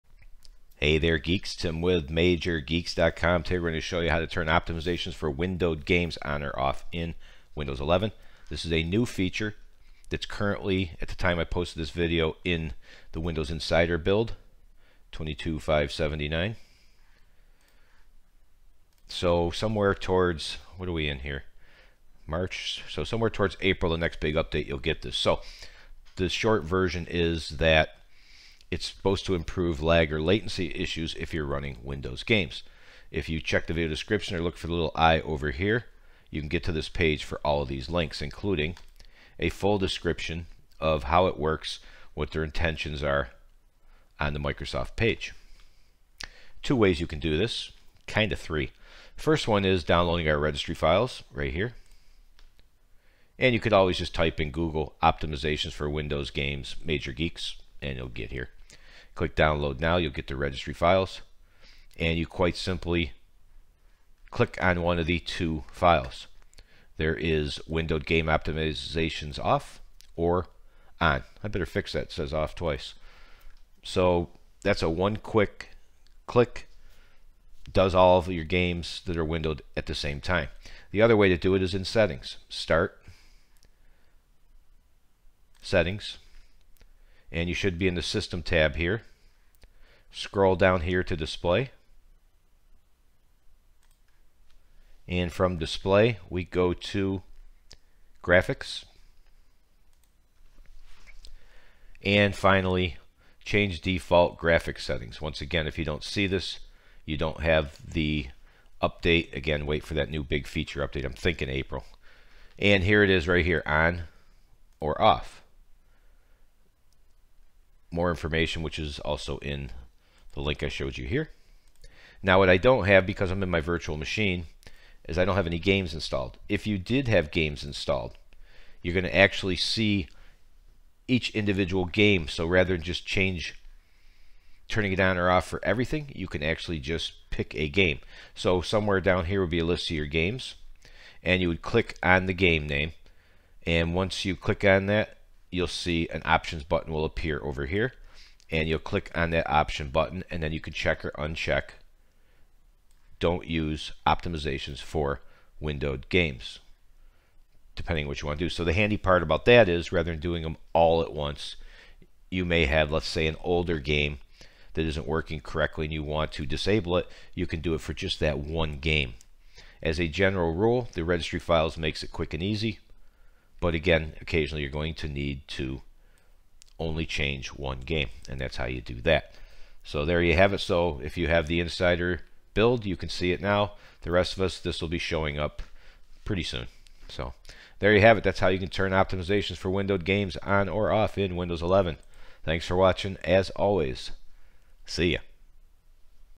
How to Turn Optimizations for Windowed Games On or Off in Windows 11
Microsoft has introduced Optimization for Windows Games in Windows 11. If you're having lag or latency issues, you can try to enable or disable Optimization for Windows Games if you are playing your games in Windowed mode.